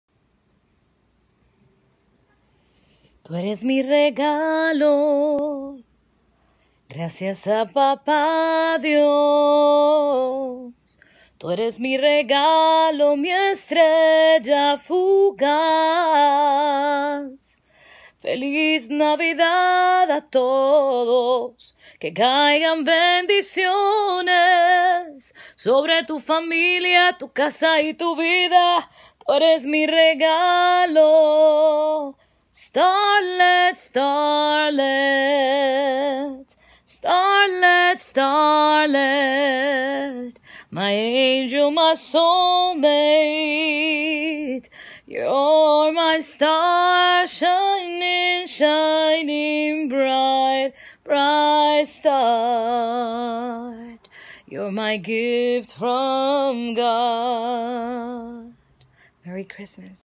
Christmas Day
Singlewarriormoms , woke up Christmas Day singing to my caramel munchkin. She was 23 days old! So blessed to be able to take care of her, love her, breastfeed her, hold her, sing for her! So many moms around the world are not as fortunate please be thankful for each second with your baby! Your babies health and please love them endlessly! Provide to them what you never had !